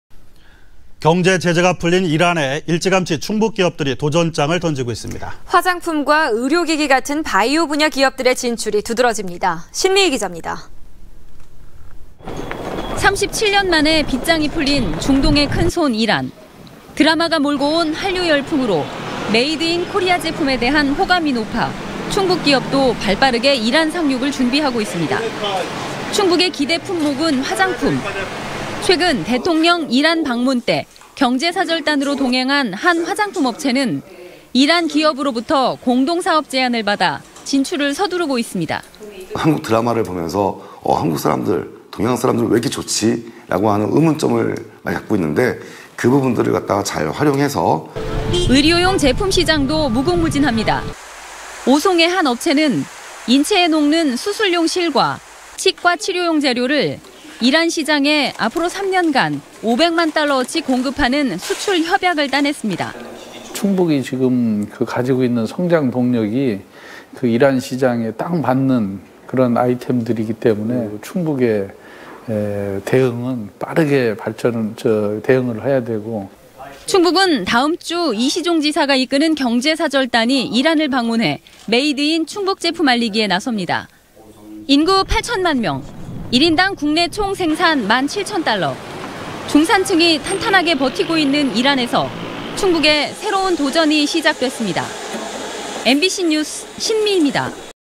[청주MBC뉴스] 이란 시장을 잡아라
◀앵커▶
 경제 제제가 풀린 이란에 일찌감치
충북 기업들이 도전장을 던지고 있습니다.
 화장품과 의료기기 같은 바이오 분야
기업들의 진출이 두드러집니다.
 신미이 기자입니다.

           ◀ＥＮＤ▶

  37년 만에 빚장이 풀린 중동의 큰손 이란.

 드라마가 몰고 온 한류 열풍으로
메이드인 코리아 제품에 대한 호감이 높아,
충북 기업도 발빠르게 이란 상륙을
준비하고 있습니다.

 충북의 기대 품목은 화장품.

 최근 대통령 이란 방문때 경제사절단으로
동행한 한 화장품 업체는
이란 기업으로부터 공동사업 제안을 받아
진출을 서두르고 있습니다.

           ◀ＩＮＴ▶
     김태곤 대표/파이온텍

 의료용 제품 시장도 무궁무진합니다.

 오송의 한 업체는 인체에 녹는 수술용실과
치과 치료용 재료를 이란 시장에
앞으로 3년간 500만 달러어치 공급하는
수출협약을  따냈습니다.

           ◀ＩＮＴ▶
 오석송 회장/메타바이오메드

충북은 다음주 이시종 지사가
이끄는 경제사절단이 이란을 방문해,
메이드인 충북 제품 알리기에 나섭니다.

인구 8천만명,
1인당 국내총생산 만7천 달러,
중산층이 탄탄하게 버티고 있는 이란에서
충북의 새로운 도전이 시작됐습니다.
 MBC뉴스 신미이입니다.